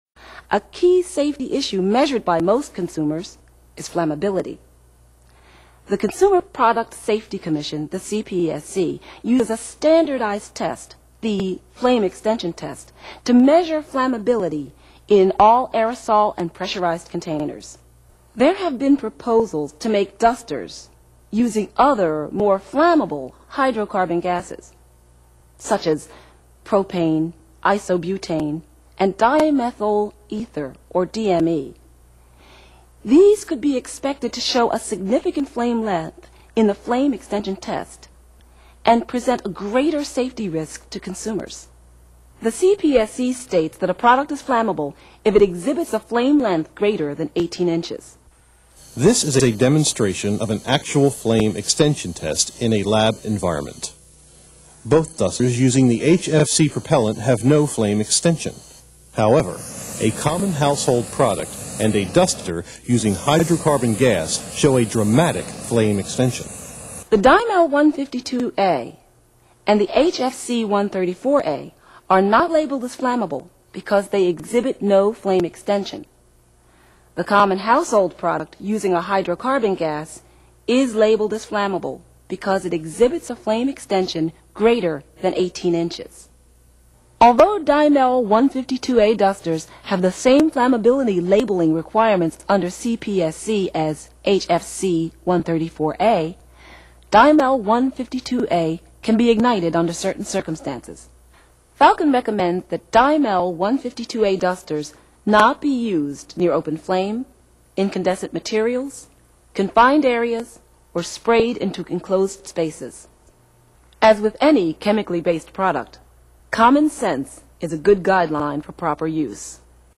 Flammability of Dusters Overview
While none of Falcon's dusters can be ignited during normal use, Dust-Off® General Use dusters (dusters containing Dymel 152A - Difluoroethane) should NOT be used near potential ignition sources, such as open flames, hot surfaces, or spark producing equipment (such as paper shredders).

If a duster is a preferred cleaning tool in an area where flammability is a concern, it is HIGHLY RECOMMENDED that a Dust-Off® Special Application duster (dusters that contain Dymel 134A - Tetrafluoroethane) be used. Our Special Application dusters, and all dusters containing pure 134A, are completely non-flammable.